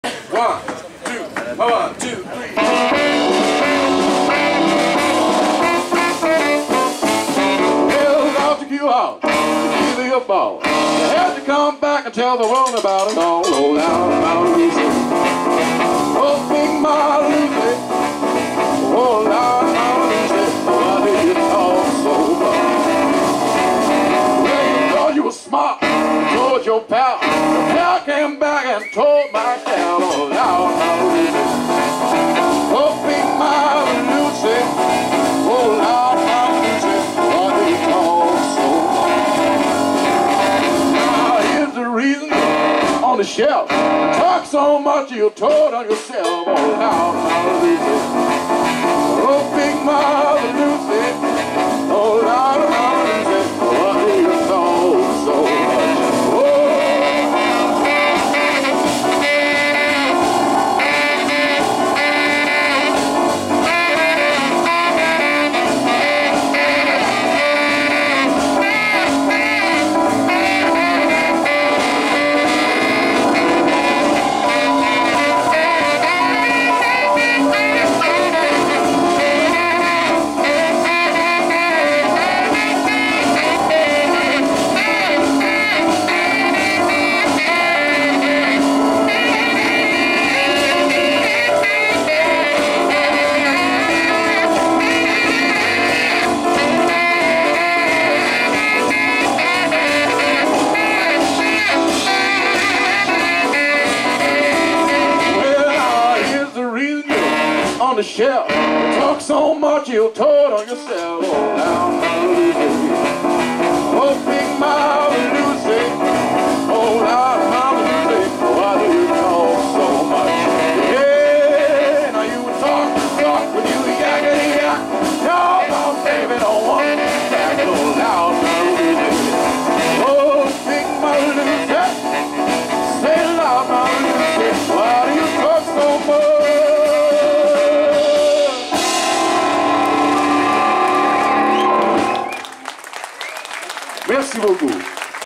Cat & Agusti : Unknown song 4
Agusti  Burriel : vocal
Cat Lee King : Piano, vocal
Stéphane Barral : Double bass
Mat Le Rouge : Tenor sax
Simon "Shuffle" Boyer : Drums
Retro Rockin' Mont Dore, scène principale